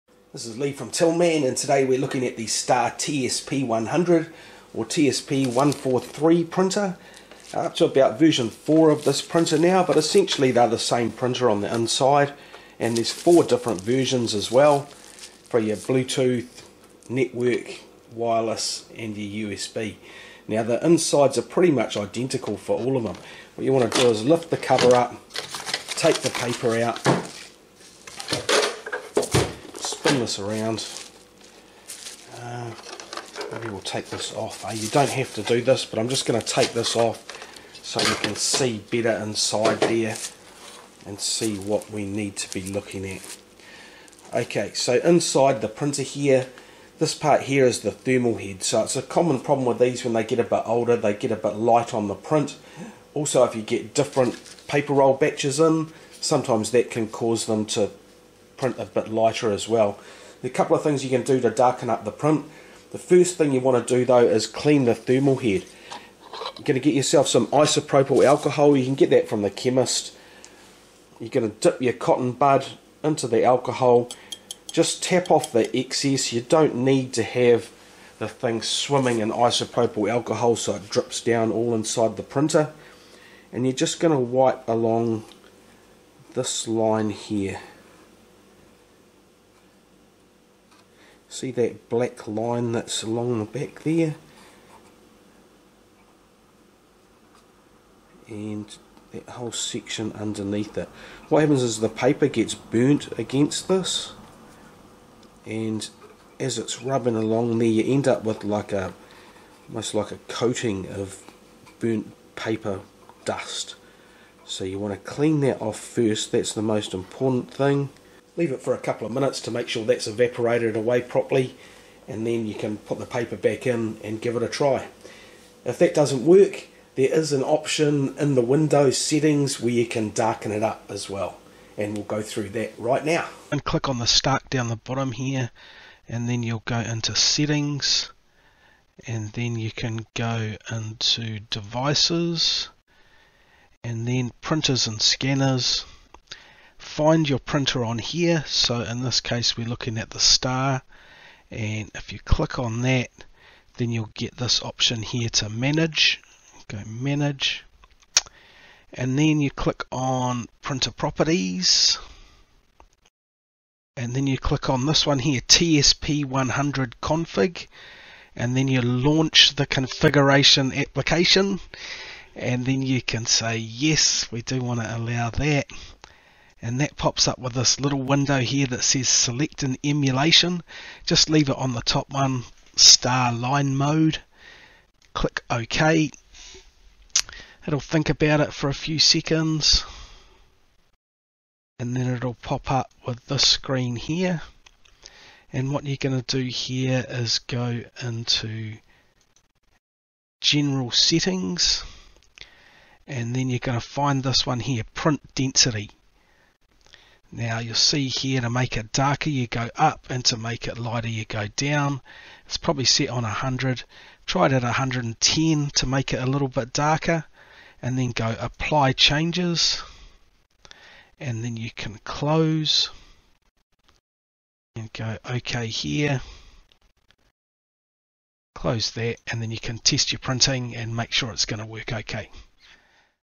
How to darken the printing on a Star TSP100 or TSP143 Receipt Printer
How to clean the thermal head and adjust the windows settings to darken the printing on a Star TSP143 (TSP100) Receipt Printer. This works for all models and versions of the Star Micronics Receipt Printer.